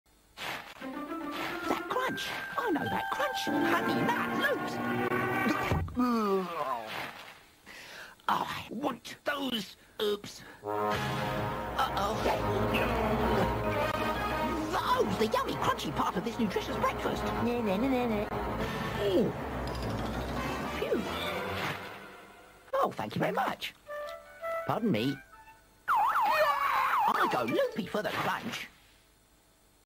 1993 Honey Loops Crunch Advert
Classic Kelloggs Ad Commercial featuring Barnabee